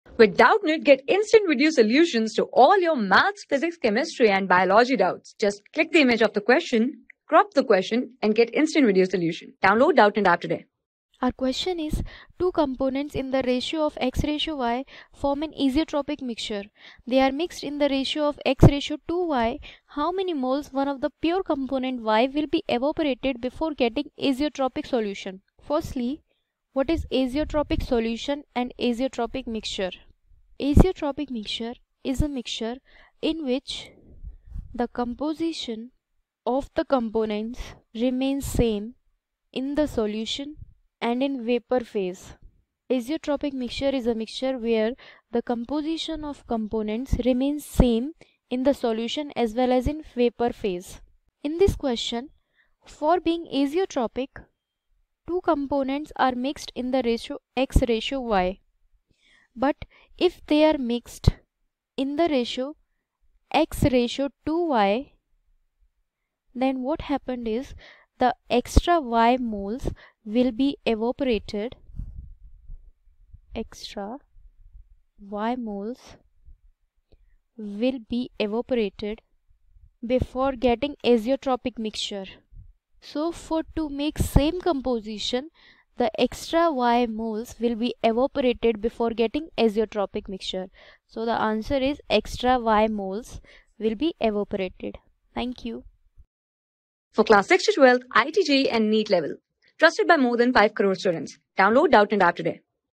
Two components in the ratio of x:y form an azeotropic mixture. They are mixed in the ratio of x:...
Two components in the ratio of x:y form an azeotropic mixture. They are mixed in the ratio of x:2y, how many moles one of the pure component y will be evaporated before getting azeotropic solution ?
Class: 12
Subject: CHEMISTRY
Chapter: SOLUTIONS
Board:IIT JEE

👉 Have Any Query? Ask Us.